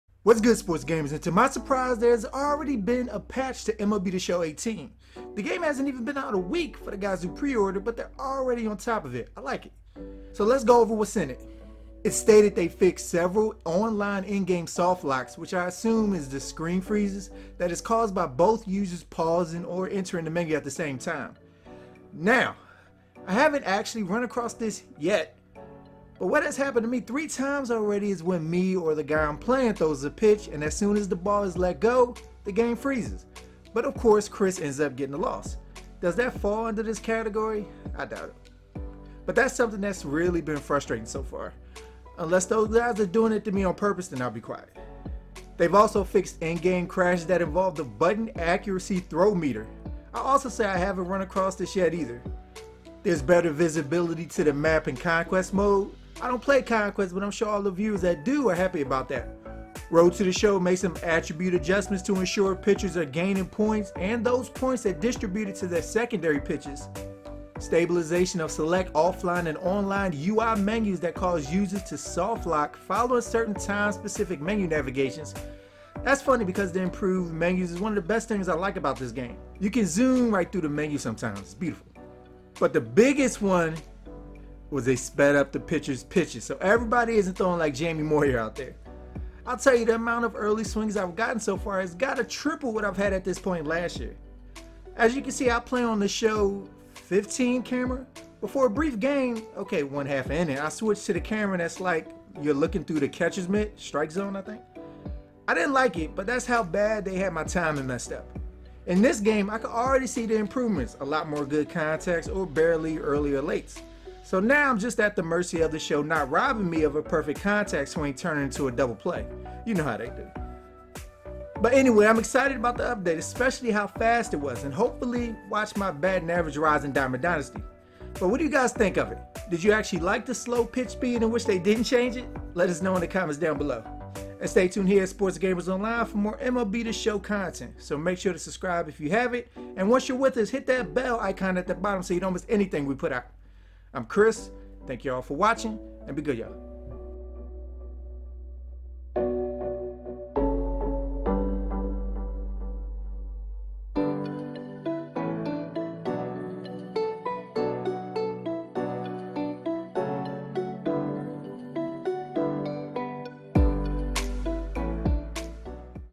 MLB The Show 18 Patch 1.05 Details
MLB The Show 18 Patch 1.05 Details. FULL DETAILS

MLB The Show 18 is an upcoming Major League Baseball (MLB) video game in development by SIE San Diego Studio and to be published by Sony Interactive Entertainment. It will be the thirteenth entry of the MLB: The Show franchise, and will be released on March 27, 2018, for PlayStation 4. Aaron Judge will be featured as the cover star for the U.S. version, while Marcus Stroman will be on the cover of the Canadian version. A limited amount of people were invited to a closed alpha, which began on December 22, 2017 until December 26, 2017 to test the online servers.

Matt Vasgersian, Dan Plesac and Mark DeRosa will be the play-by-play commentators. DeRosa replaces Harold Reynolds, who will not return after one season with The Show. Mike Carlucci returns as public address commentator.